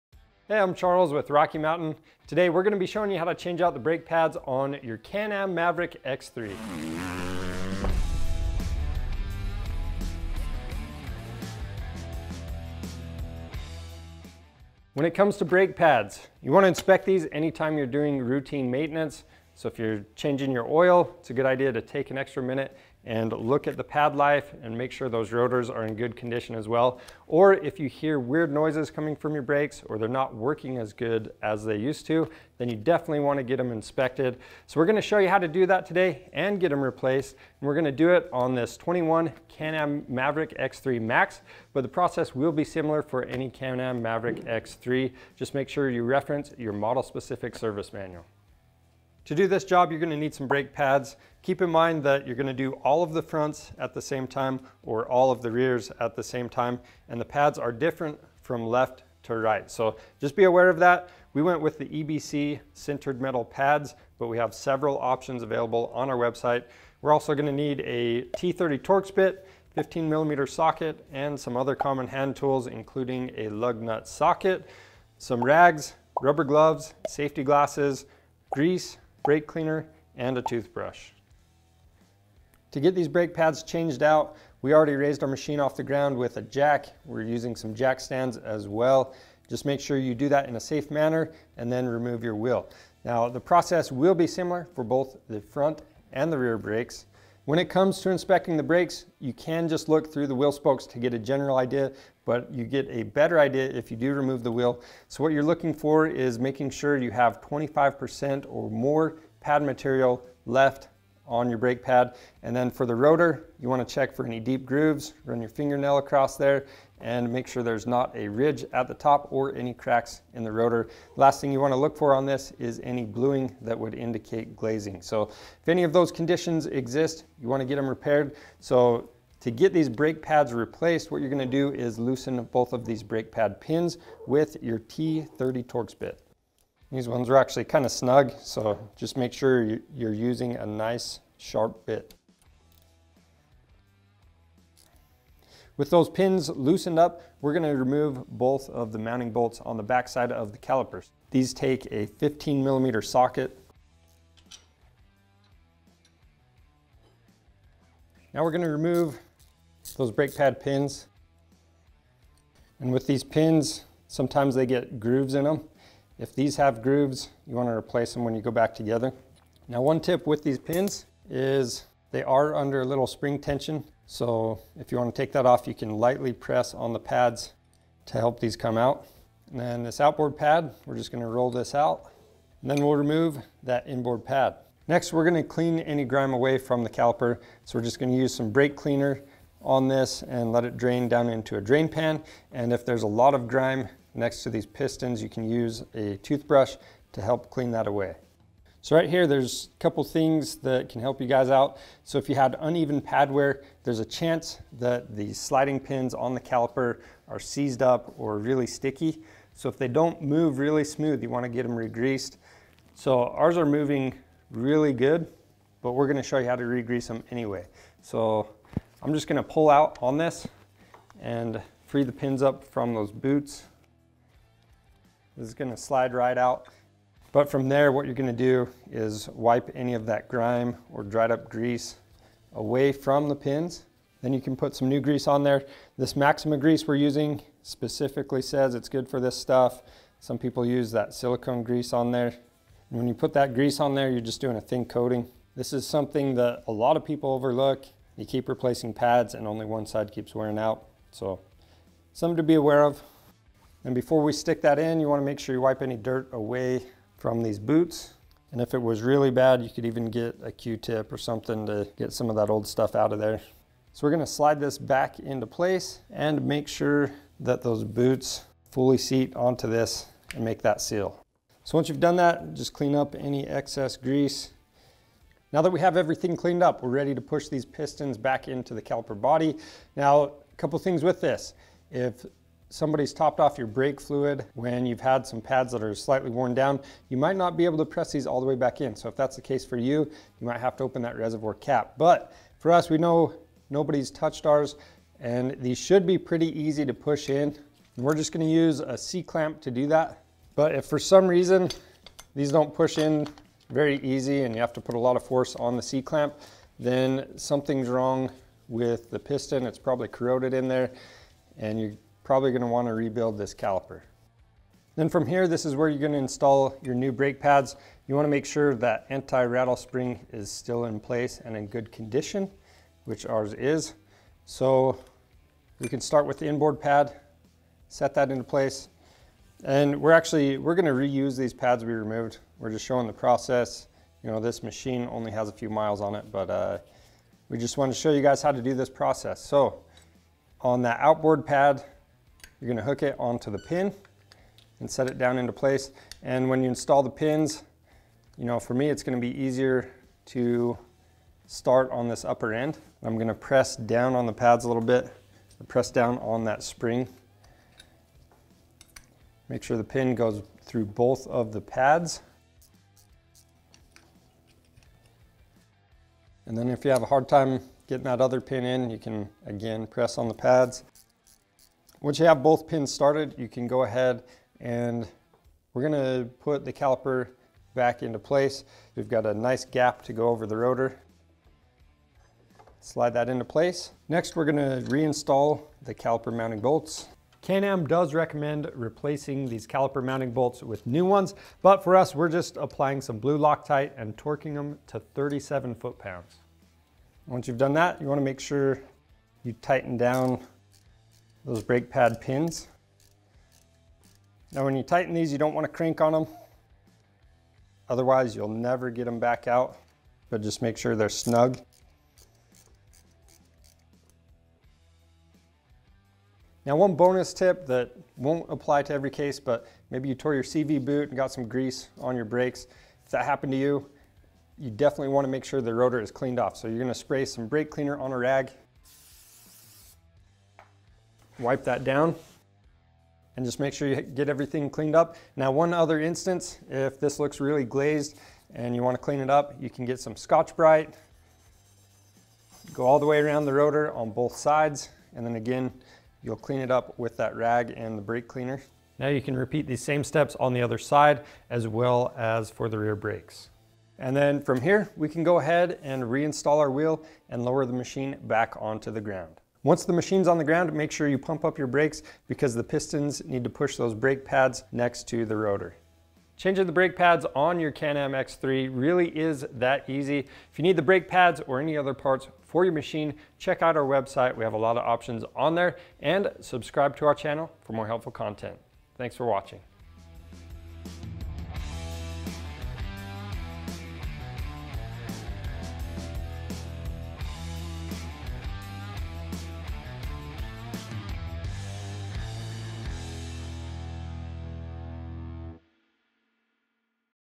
How To Replace the Brake Pads on a Can-Am Maverick X3
One of the most overlooked but important maintenance jobs you can do for your Maverick X3 is checking and/or changing the brake pads and rotors. Having a fresh set of brake pads on your machine can give you more confidence while out on the trails. In this video, Charles shows how to properly replace brake pads on a Can-Am Maverick X3.